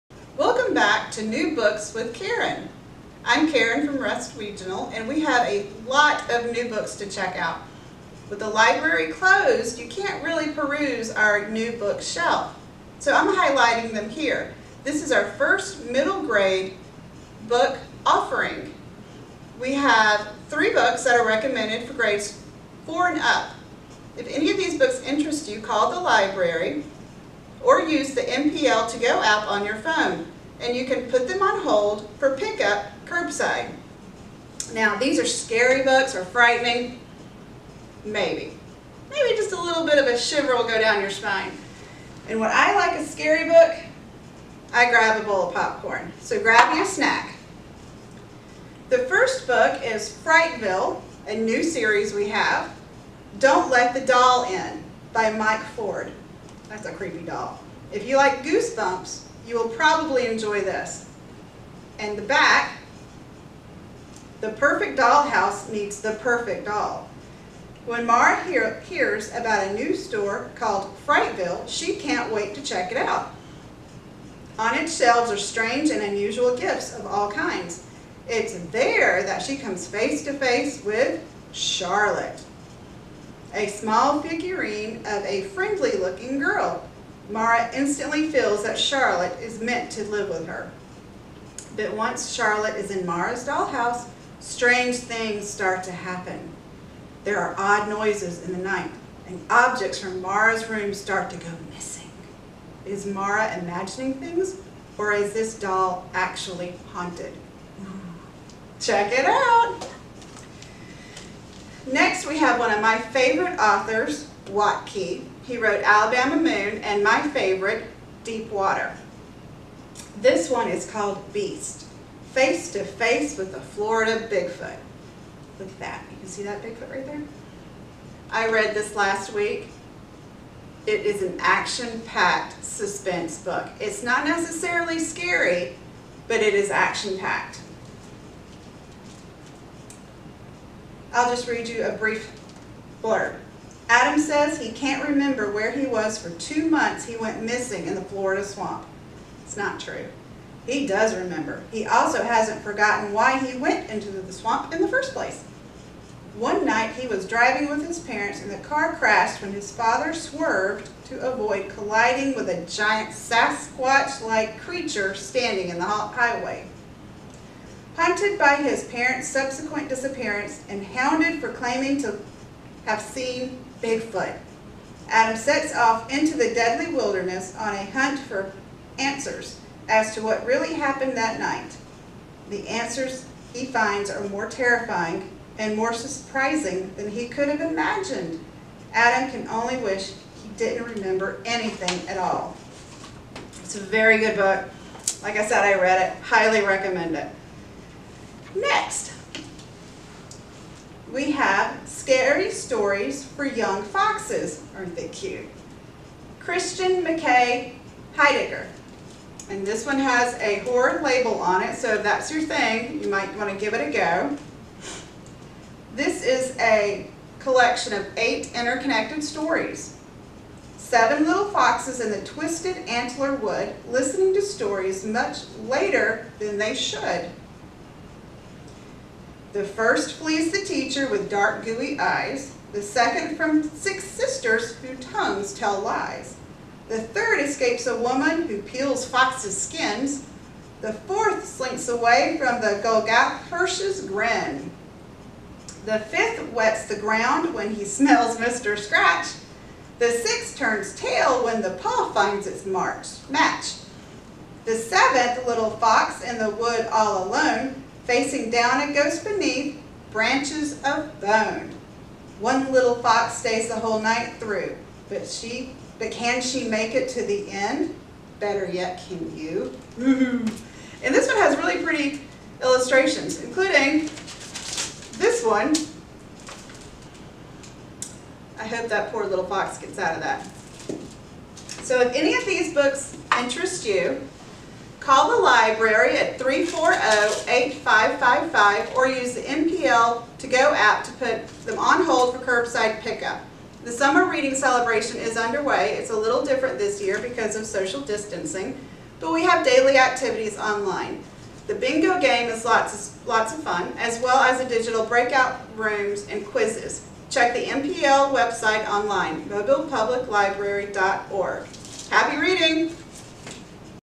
New Books with Caren: 4th Grade & Up
Attention Parents!  Don't know what books to check out for your child?  Ms. Caren has some suggestions.